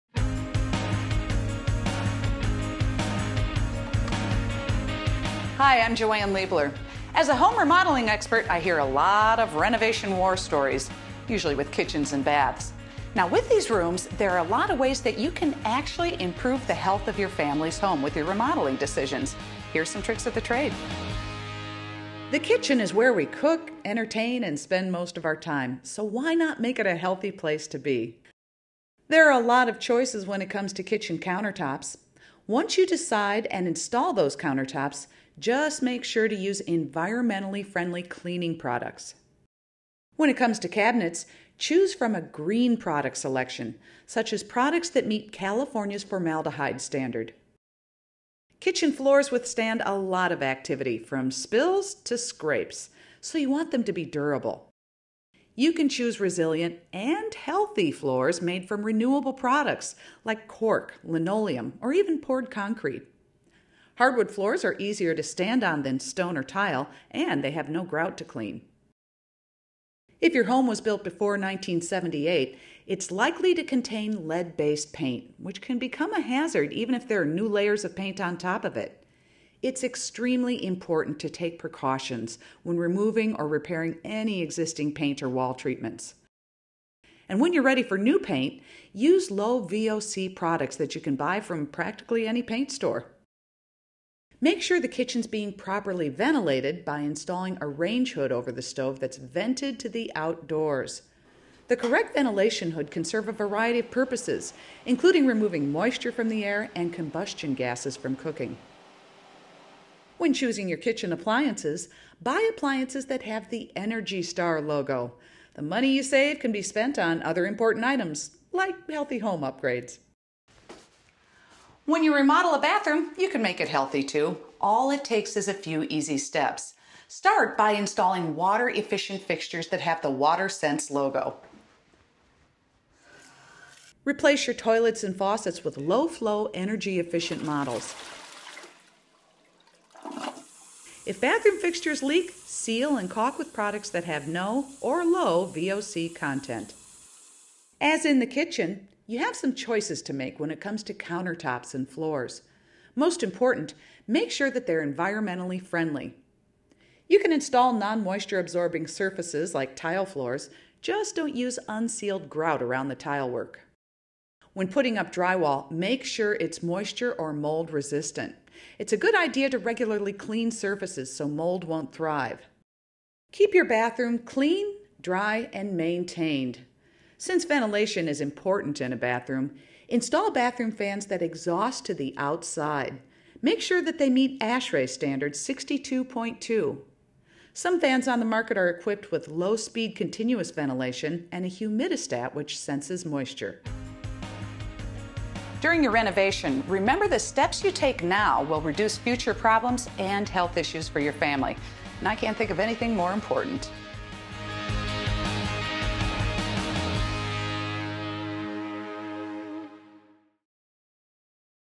Healthy Kitchen and Bathroom Remodeling with JoAnne Liebeler
Original broadcast date: September 15, 2011

About JoAnne Liebeler: A nationally recognized home improvement expert, JoAnne "JoJo" Liebeler has hosted four home improvement and design television series, "Hometime," "Home Savvy," "Room for Change," and "Passport to Design." As a producer and project manager, she has served as an executive producer on  "Passport to Design" for the Travel Channel, developed and pitched television shows, produced and written programs, and managed dozens of video projects. Her credits include History Channel, TLC, HGTV, PBS, DIY, Fox, and the Travel Channel. Ms. Liebeler's home improvement expertise was tapped by Sunset Books when she was invited to author "JoAnne Liebeler’s Do It Herself: Everything You Need to Know to Fix, Maintain, and Improve Your Home" and contributed to "Big Ideas for Small Spaces." Early in her career, Ms. Liebeler  also appeared in the film "The Glass Shield" and in the sitcoms "Roseanne" and "Sister Sister." In addition to her work on NCHH's board from 2007 to 2018, Ms. Liebeler has also served the president of Minnesota Women in Film and Television (MN WIFT).